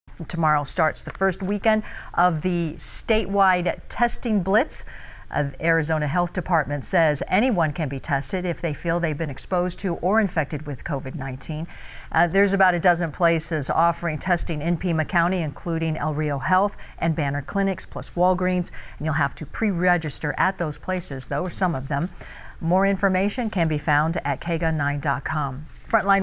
Thousands in Arizona to be tested for COVID-19 on weekends in May
Arizona officials are aiming to test tens of thousands of people for coronavirus over the next few weekends.